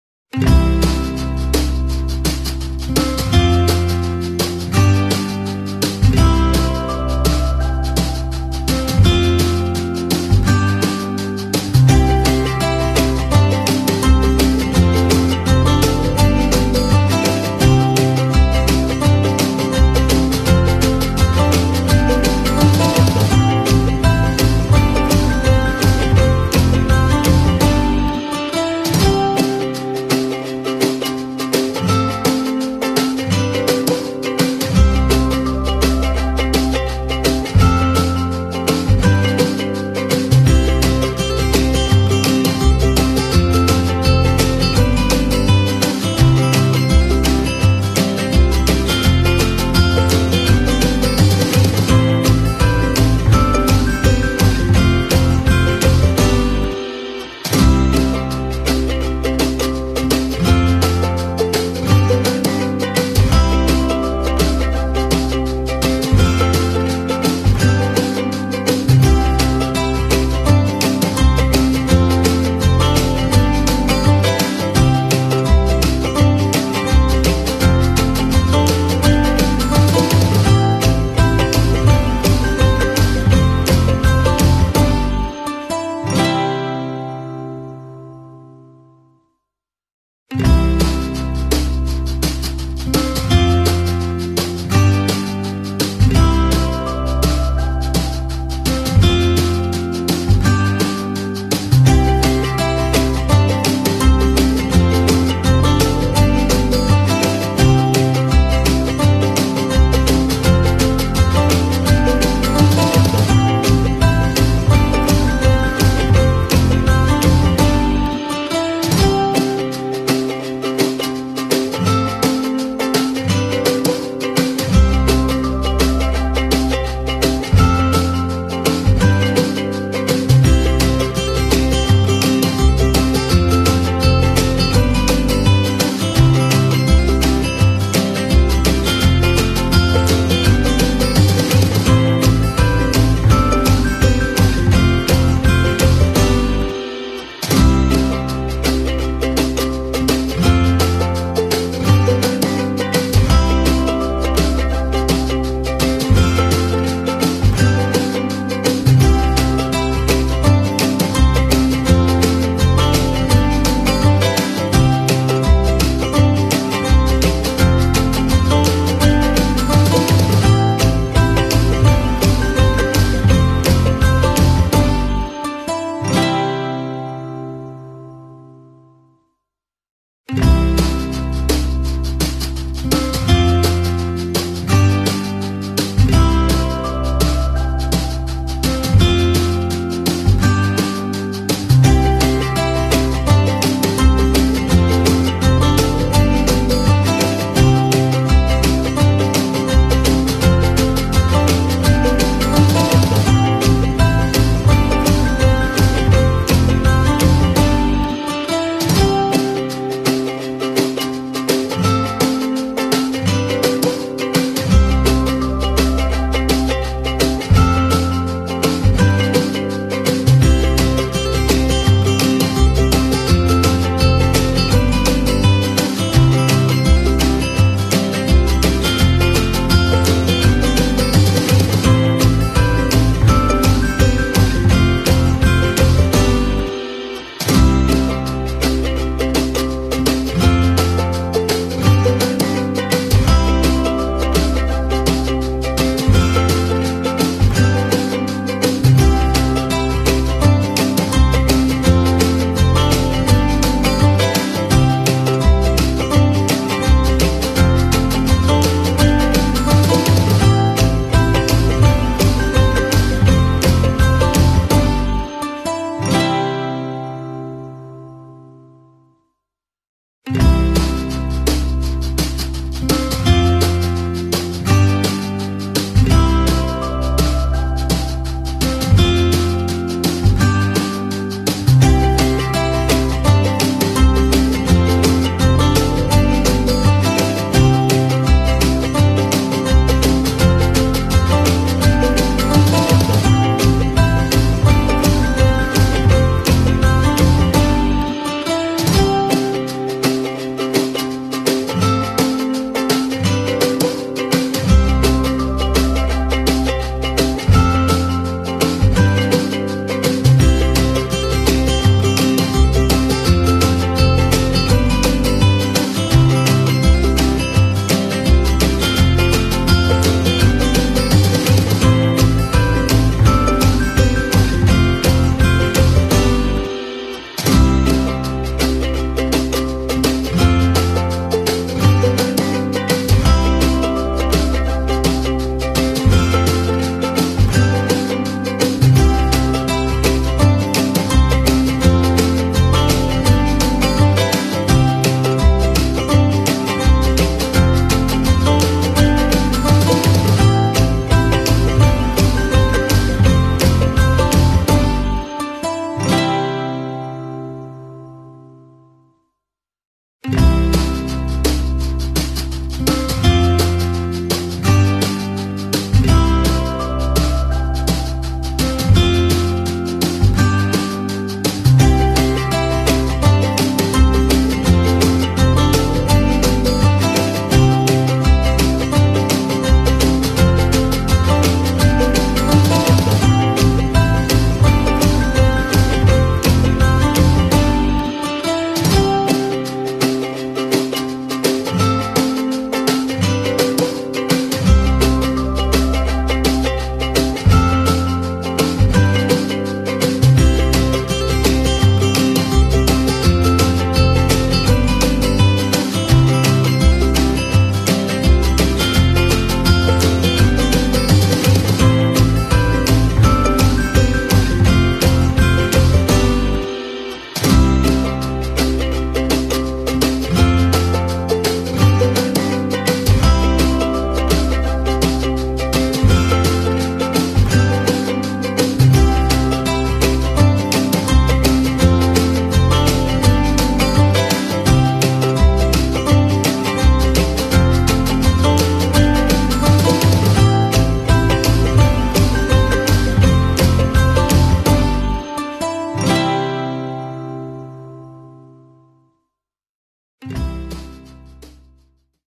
Sunday special Origami sofa chair
Sunday special How to make origami sofa chair in easy steps